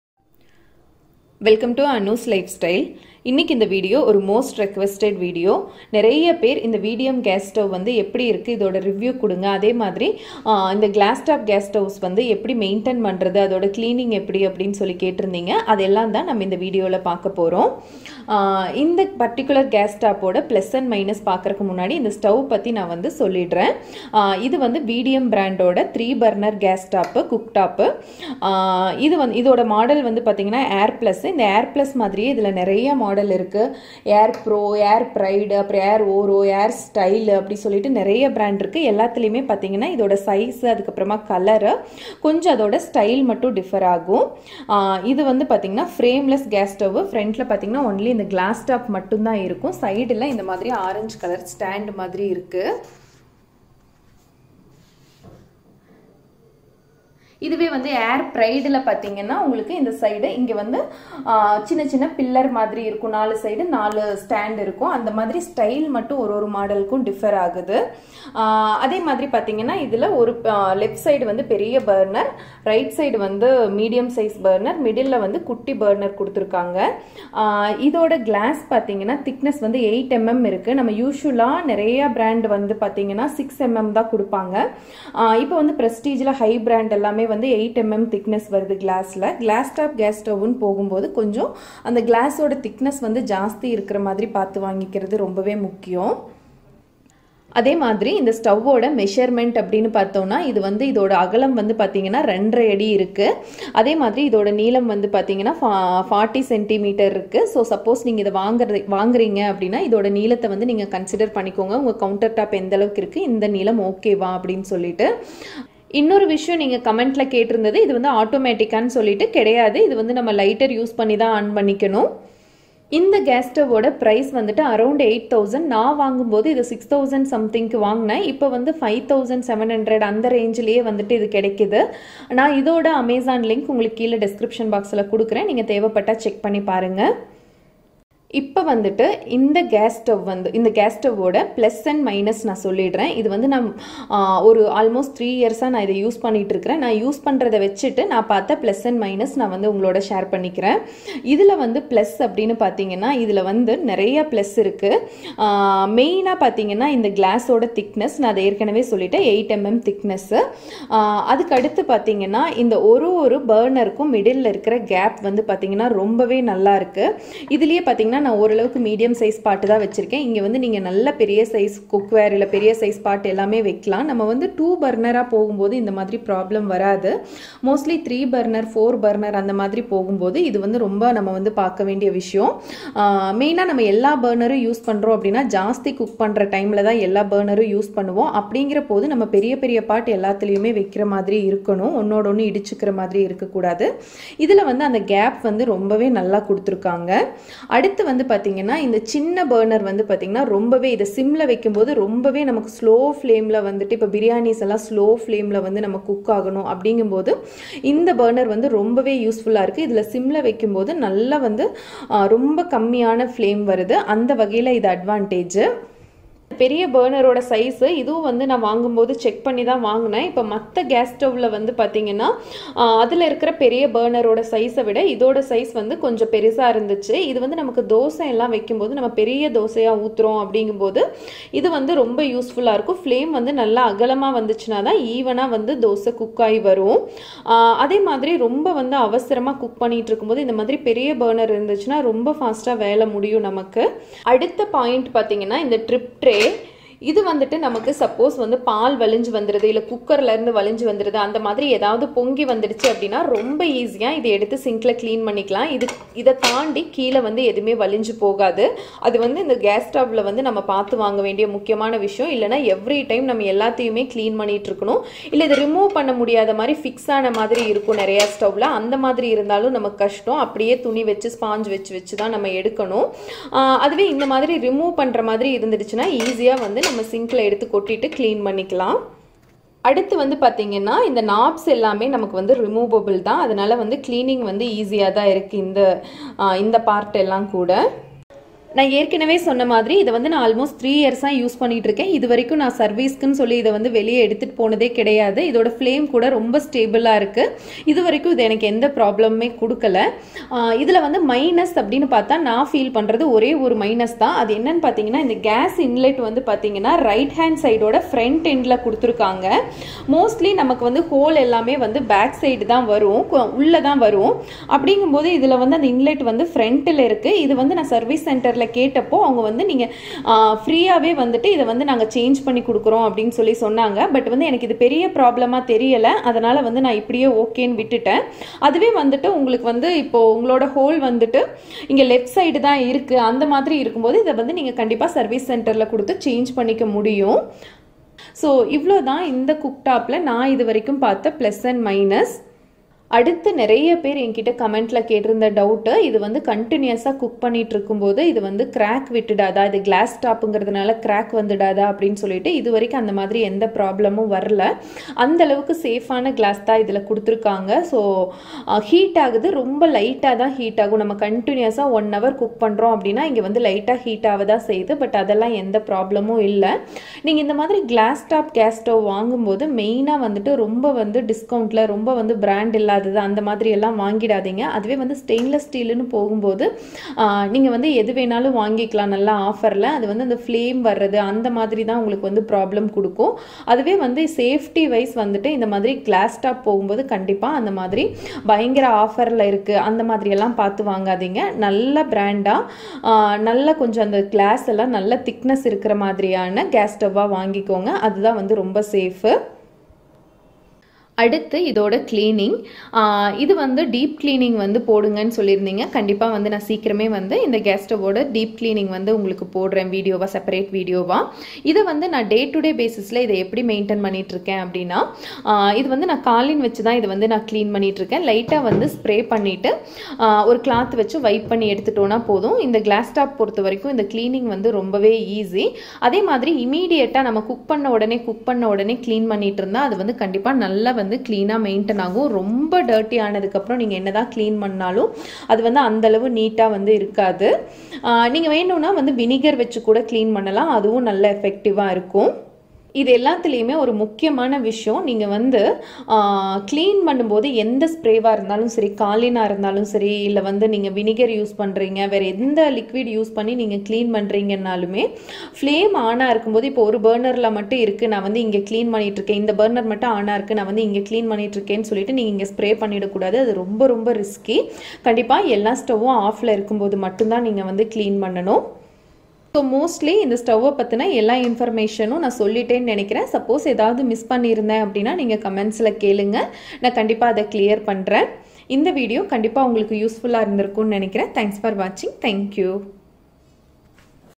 Vidiem 3 burner stove எப்படி இருக்கு? | Cleaning and Maintanance
Vidiem 3 burner stove Review , cleaning and maintenance

Vidiem Air plus 3 burner:

Vidiem Airplus 2 burner:

Hope you all liked the video. Share your suggestions in the comments below.